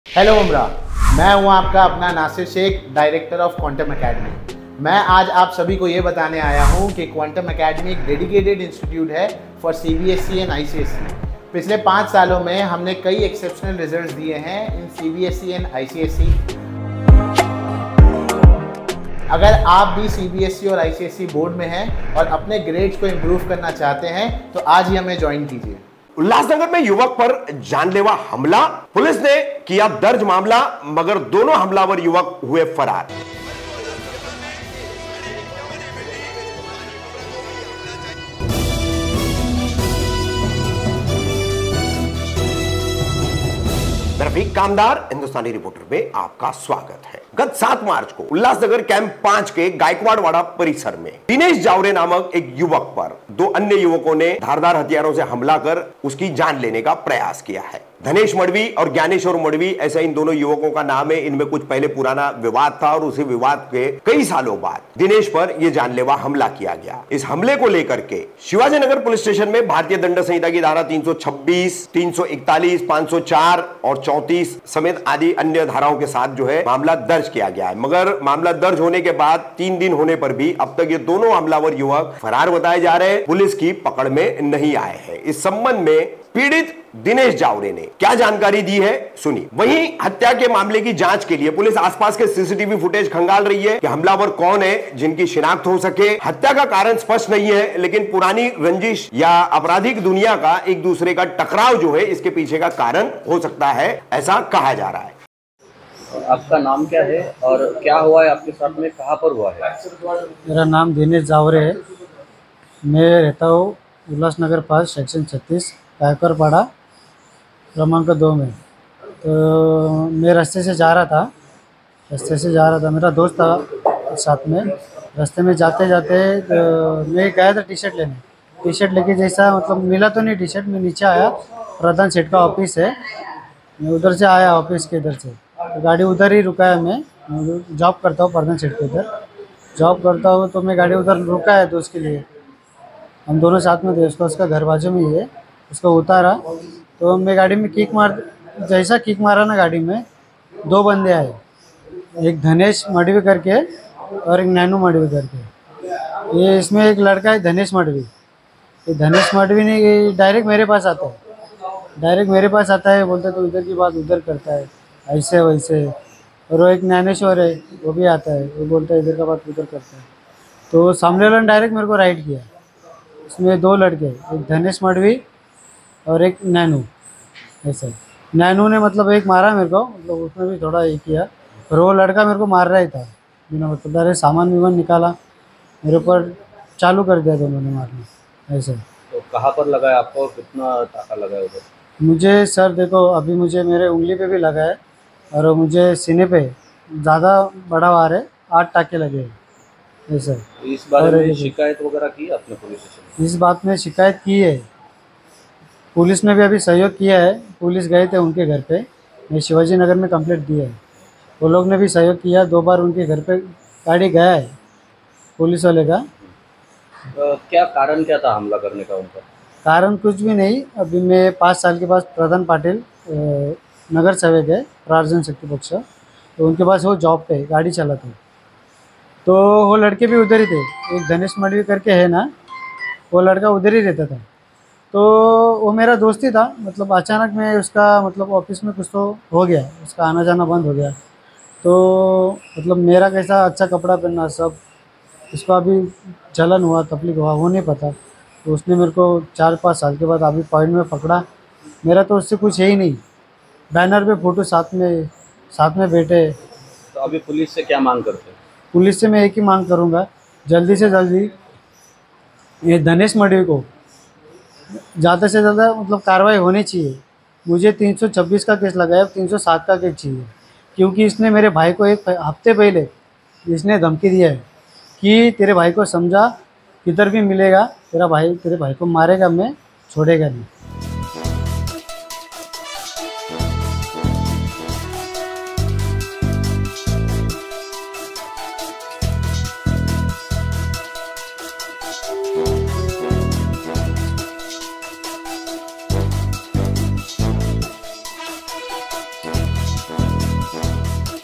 उल्हासनगर में युवक पर जानलेवा हमलापुलिस ने किया मामला दर्ज | Hindustani Reporter
उल्हासनगर में युवक पर जानलेवा हमला

पुलिस ने किया मामला दर्ज

दोनों हमलावर युवक हुए फरार